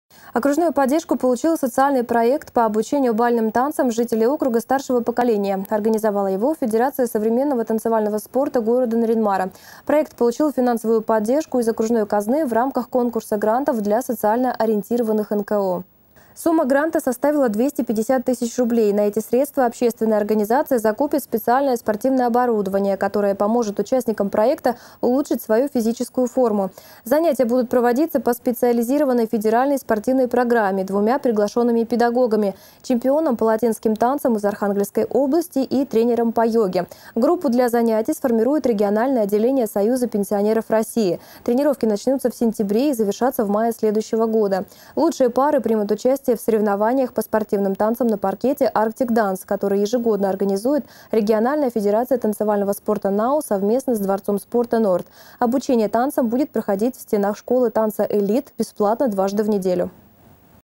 БАЛЬНЫМ ТАНЦАМ ВСЕ ВОЗРАСТЫ ПОКОРНЫ
Окружную поддержку получил социальный проект по обучению бальным танцам жителей округа старшего поколения. Организовала его Федерация современного танцевального спорта города Нарьян-Мара. Проект получил финансовую поддержку из окружной казны в рамках конкурса грантов для социально ориентированных НКО.